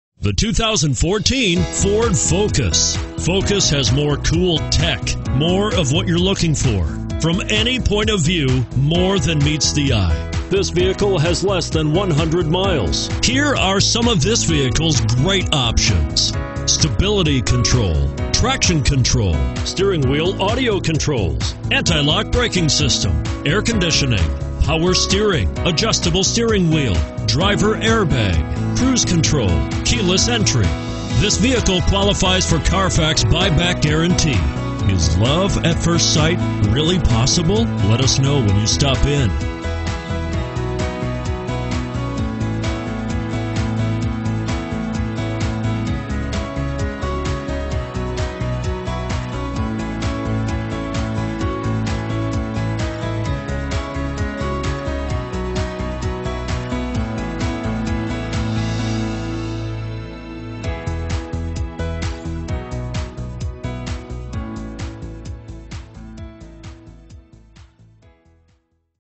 2014 Ford Focus Frankfort, Chicago, Bolingbrook P2885
2014 Ford Focus SE

This 2014 Ford Focus SE might be just the sedan for you. This is a sedan you can trust it has a crash test rating of 5 out of 5 stars! According to a review from CarsDirect, 'A new body in white employs high-strength steel for 55 percent of the structure, the highest of any U.S. Ford'.